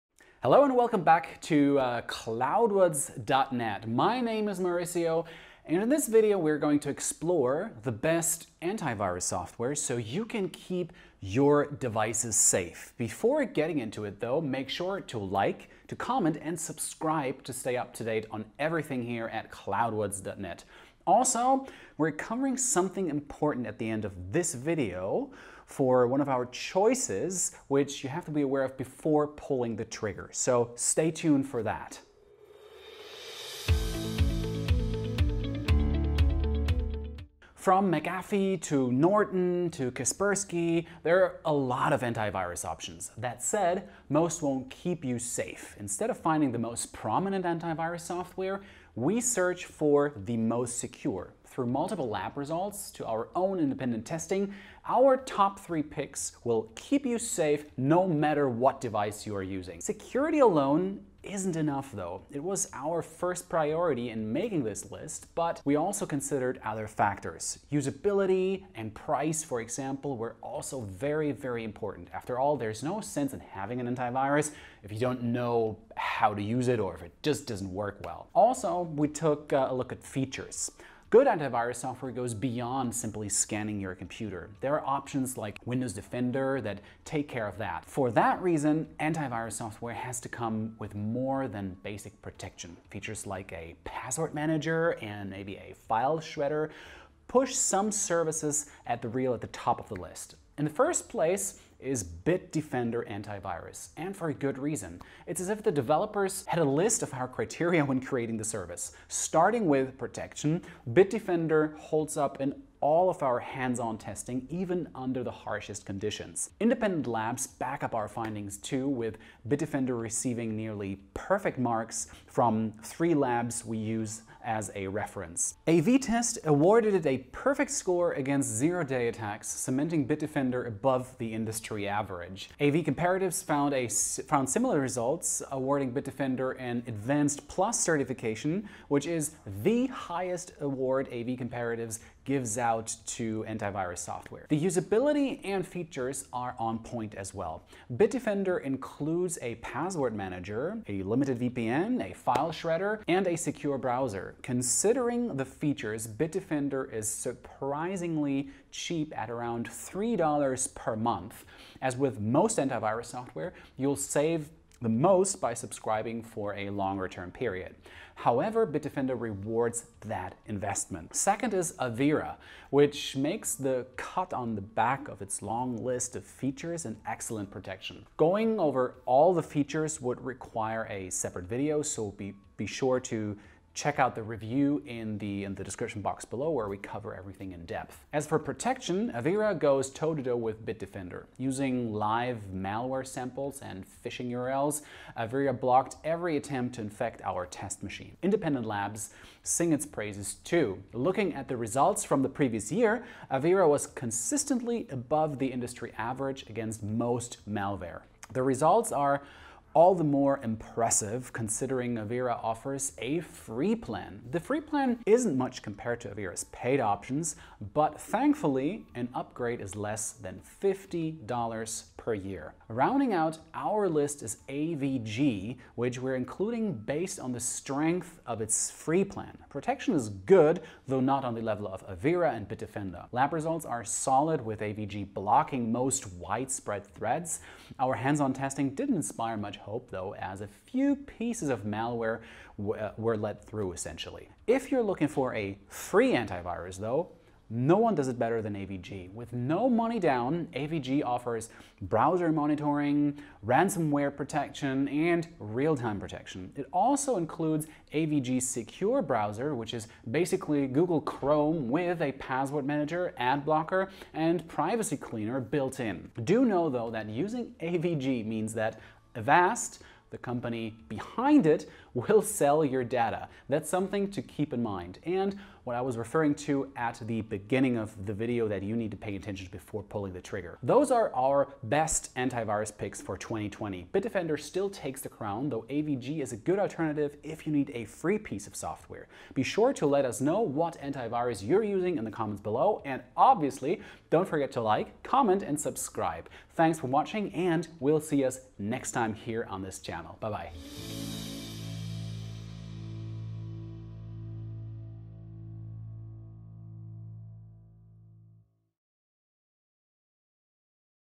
Top 3 Best Antivirus Software: What Keeps You Safe Against Malware, Viruses and Ransomware
A new year, a new roundup of the best antivirus software. We’ve gone over all the top providers, and Bitdefender is still rocking it. Join us to find out why.
0:37 Antivirus options
1:40 Bitdefender Antivirus
2:05 AV-Test - Advanced plus certification
2:24 Features
2:51 Avira
3:07 Protection level
3:29 Free plan
3:43 AVG
4:04 Best free antivirus
4:26 Beware of Avast